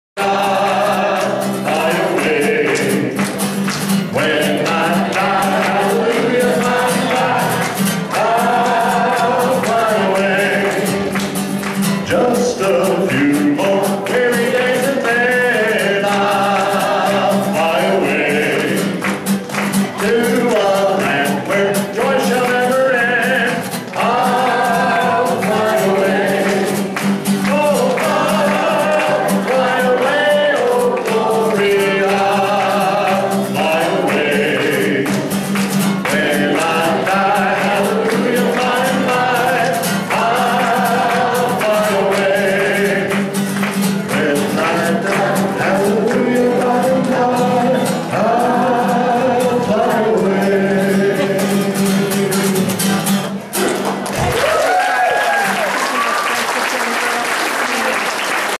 Kristyn and Mark Baker
Kristyn Harris sings with Mark Baker